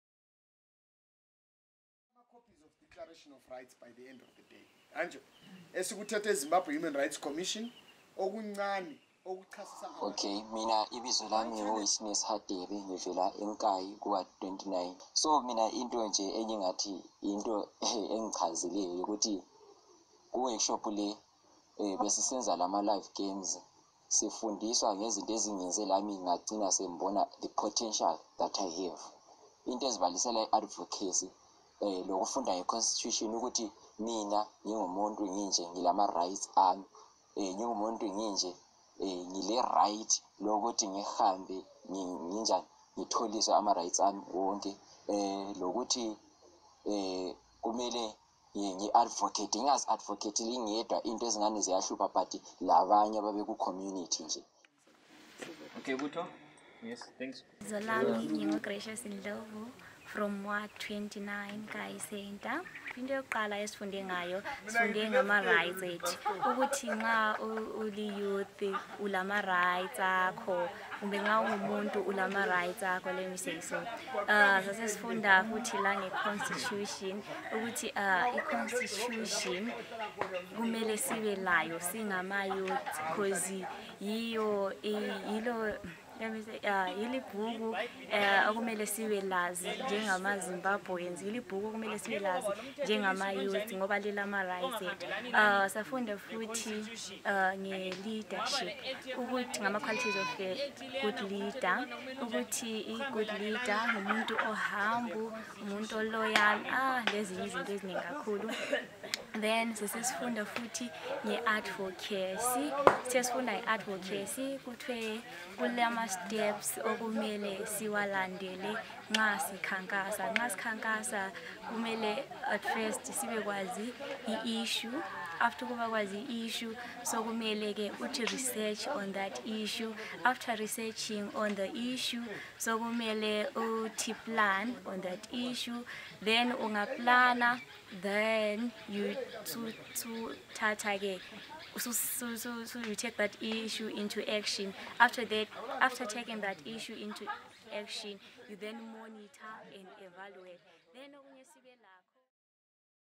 Development and Leadership Camp for Nkayi Youths.
Youths from Nkayi in Matebeleland North in Zimbabwe go through leadership and development  Camp hosted by Habakkuk Trust.